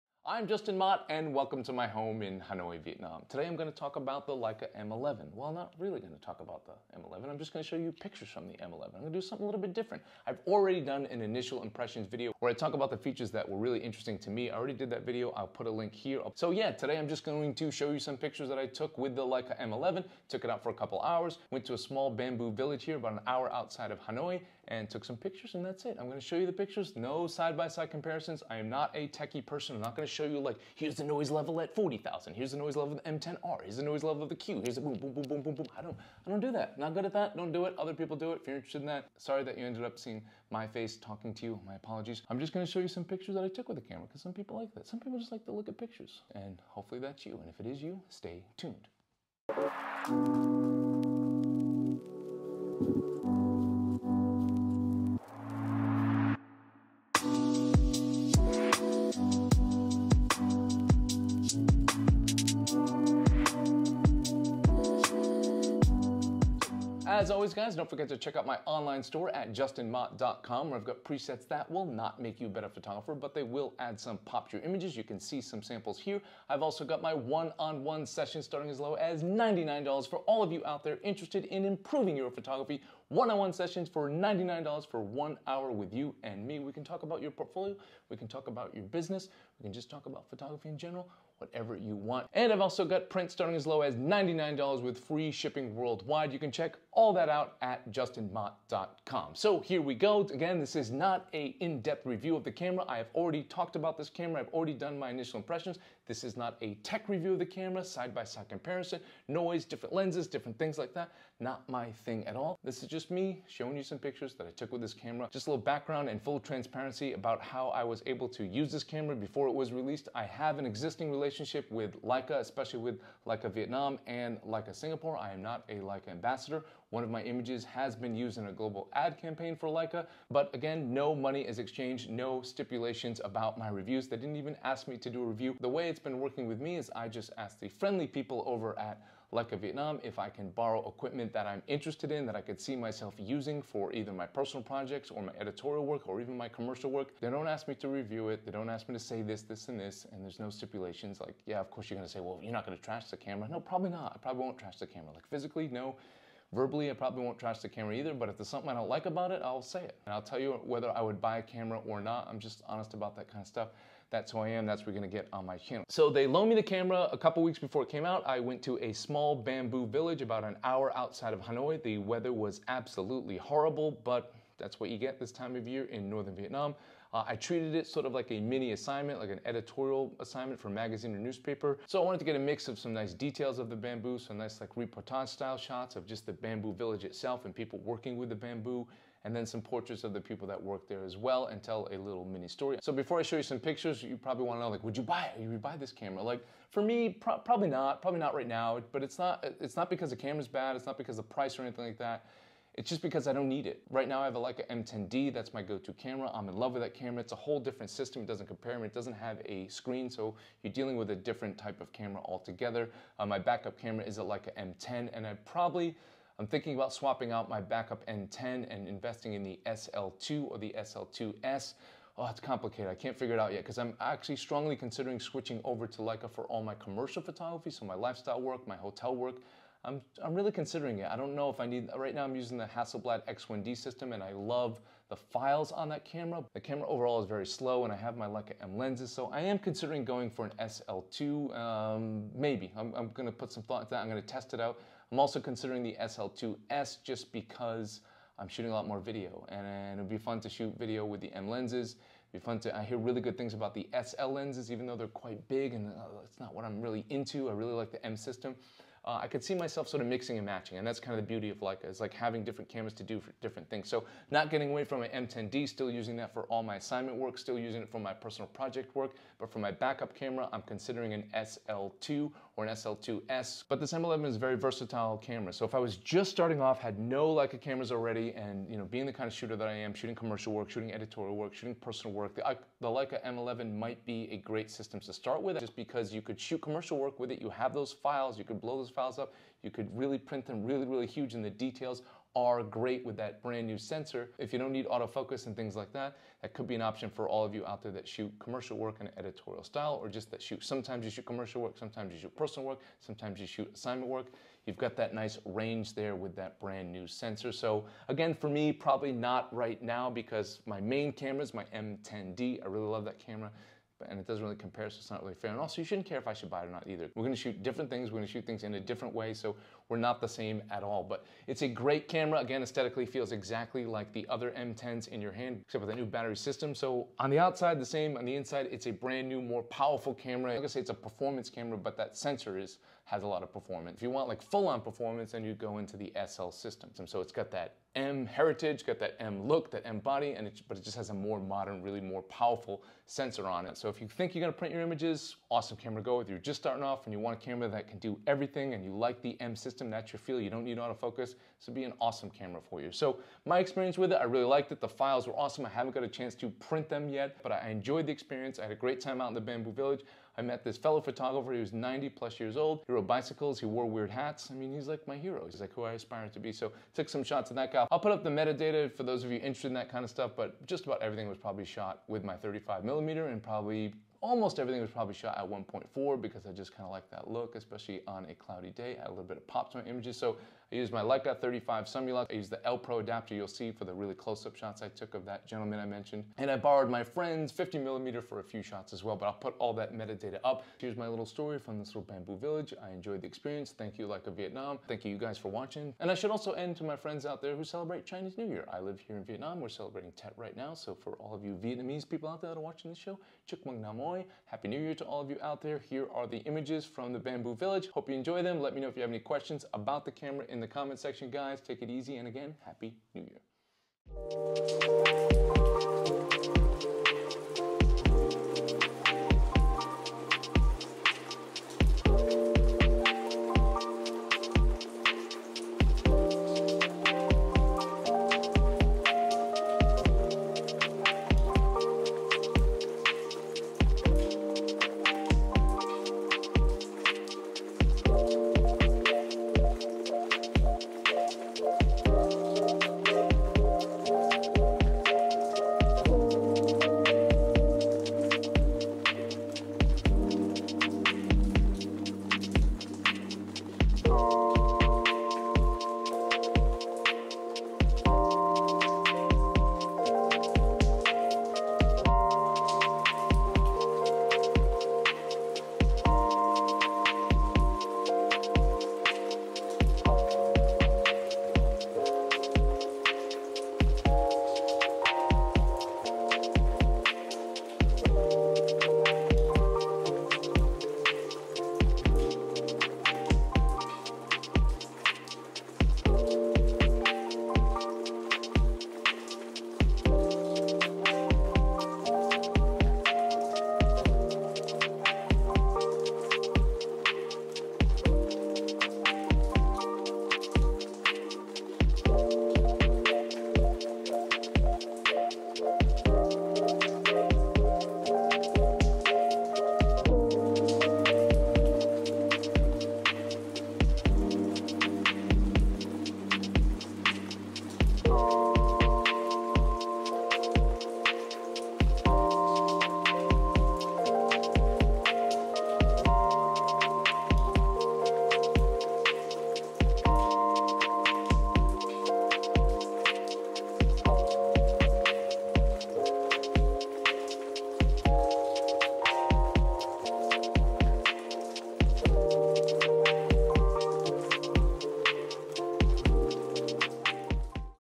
Leica M11 PHOTOS | NOT A TECHY REVIEW
In this episode, I talk briefly about the Leica M11 and whether or not I'd buy the camera along with showing some sample M11 photos I took with it at a small bamboo village near Hanoi, Vietnam.

I'll show you some untouched DNG files along with some toned JPEG files, but this isn't a techy video at all, so my apologies if that's what you are looking for, I'm not your guy for that.

TIMESTAMPS
0:00 INTRO
1:40 DISCLAIMER
3:15 WILL I BUY IT?
6:54 UNTOUCHED DNG FILES
7:23 REOUTCHED JPEG FILES